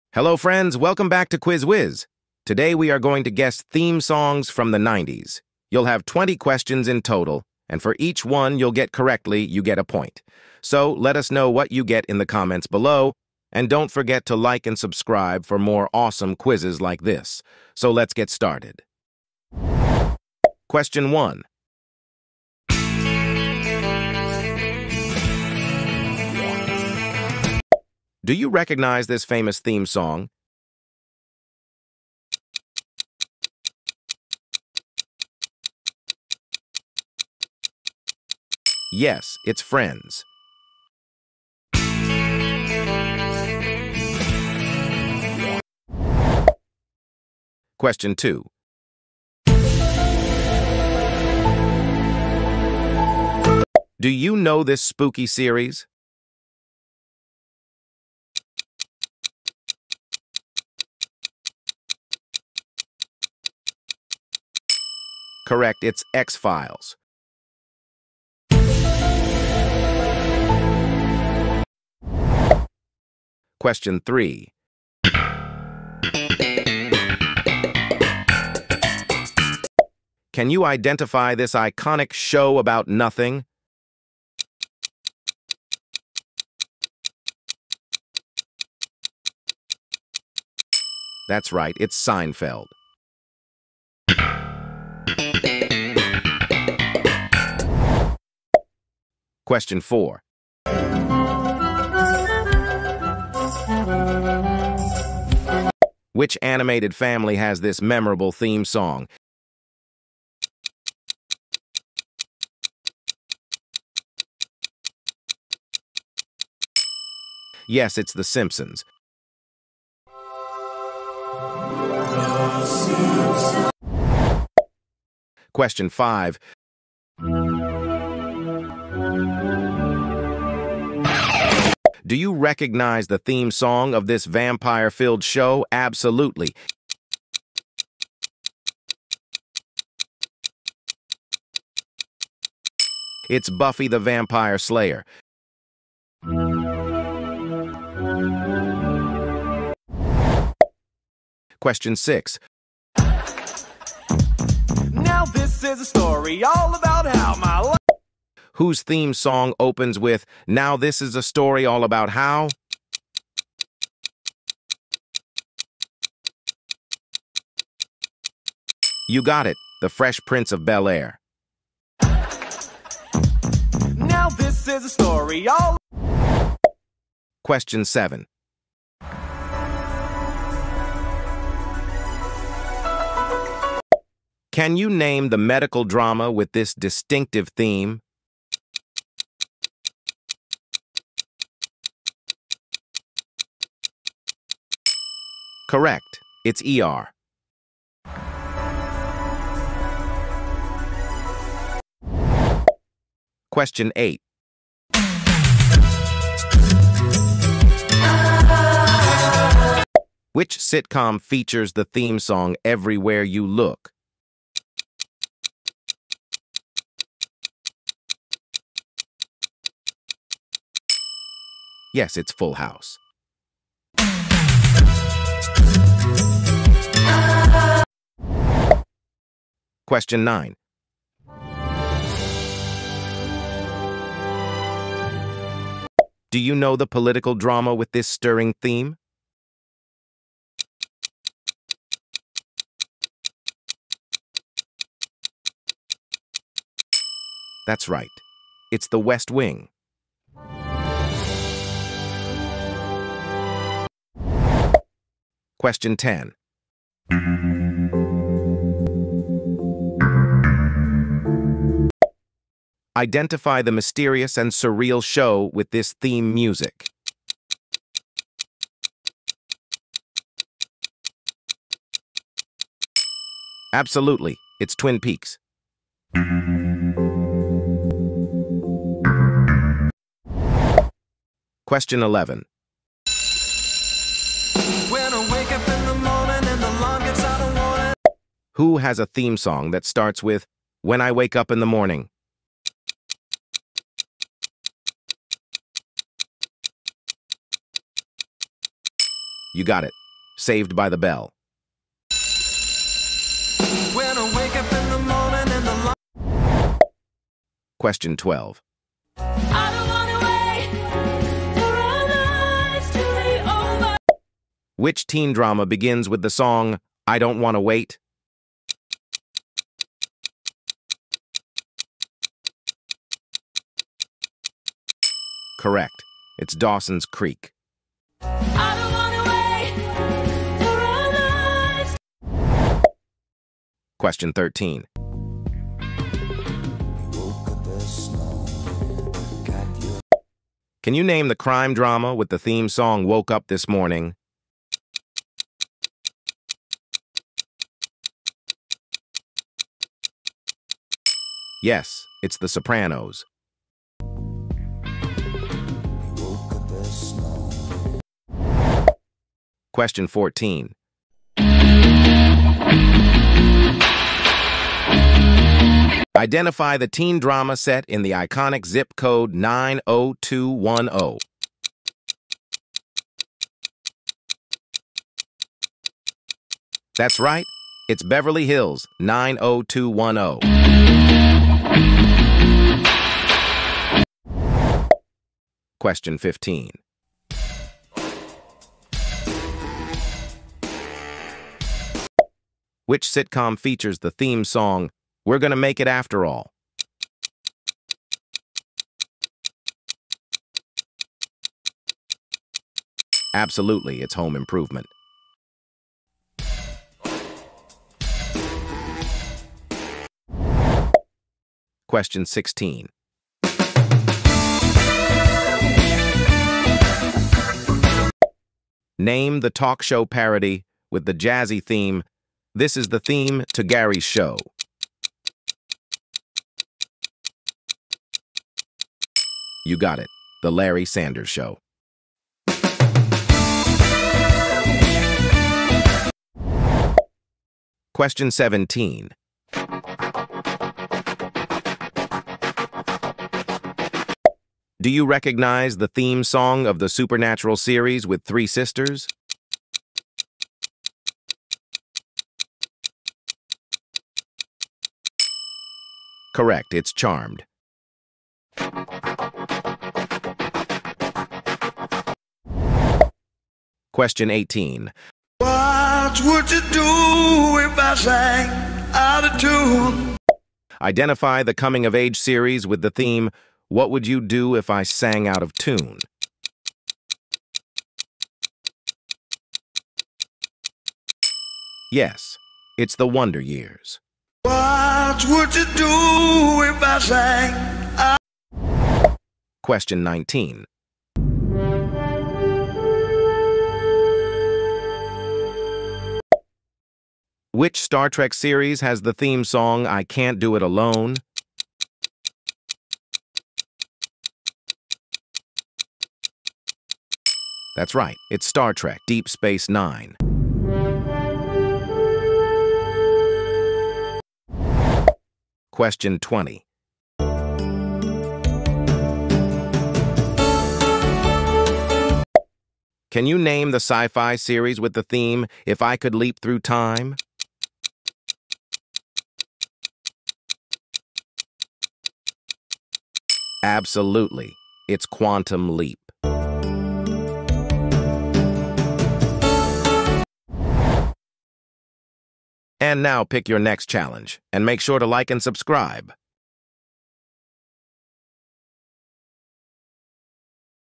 Guess the 90s TV Theme songs
📺✨ Get ready to be transported back to the golden era of television as we challenge your memory with iconic tunes that defined a generation.

🕰️ Remember the days of "must-see TV"? We've curated a collection of the most unforgettable theme songs from beloved 90s shows that will have you singing along and reminiscing about the good old days. From sitcoms to dramas, sci-fi to teen angst, this quiz covers it all!

🎶 Can you identify the show just by hearing a few seconds of its theme song? Test your knowledge and see if you can name them all! Whether you were a die-hard fan glued to the TV every week or you're discovering these classics for the first time, this quiz is the perfect blend of nostalgia and entertainment.

💡 Join us for a trip down memory lane, celebrate the unforgettable tunes that still linger in our hearts, and challenge your friends to see who truly rules the 90s TV theme song kingdom. Share your scores in the comments and let the reminiscing begin!

🚀 Hit play now and embark on a journey back to the 90s - where TV themes were catchy, shows were iconic, and the memories are timeless. Don't miss out on the fun, click play, and let's see who the true 90s TV theme song experts are! 🌈🎉